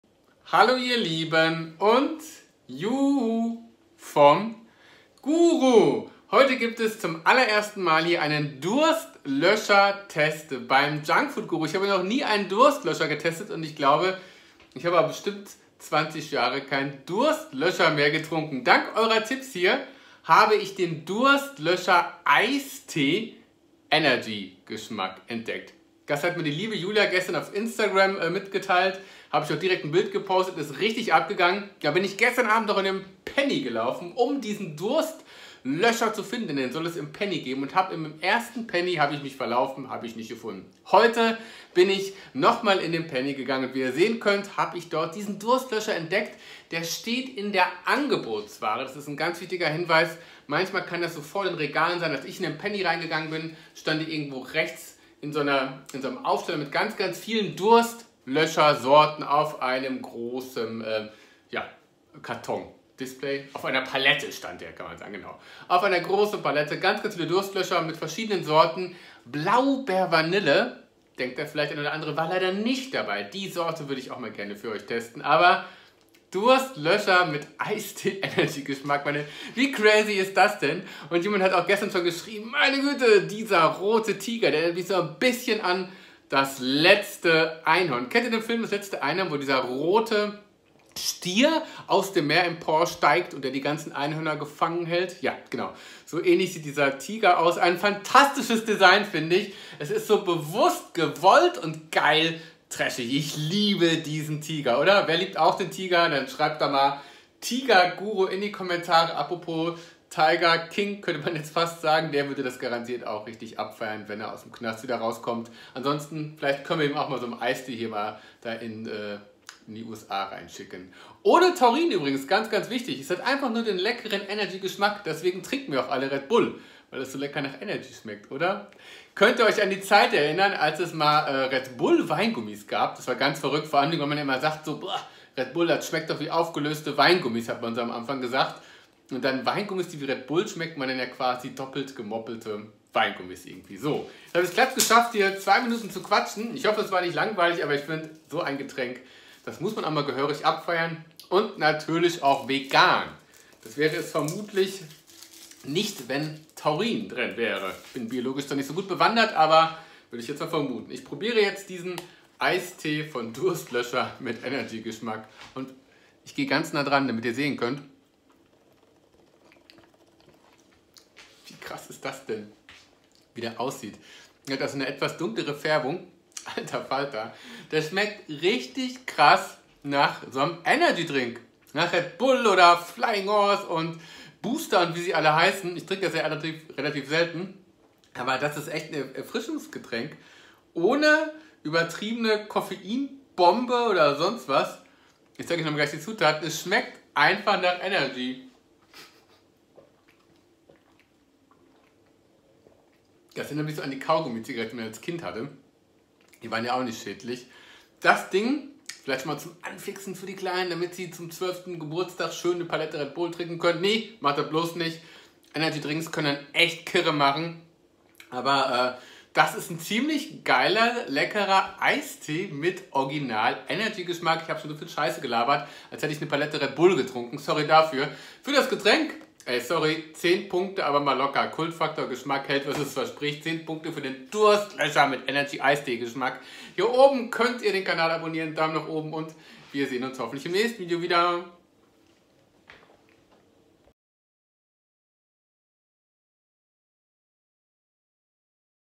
Durstlöscher ENERGY im Test: So schmeckt der Eistee mit Energy Geschmack ohne Taurin und Co!
Wie krass ist das denn? Beim Durstlöscher Energy handelt es sich um eine neue Sorte, die wie ein Energydrink schmecken soll, aber dabei kein Taurin oder sonstige typischen Energy-Inhaltsstoff hat, sondern einfach nur ein Eistee mit dem entsprechenden Geschmack.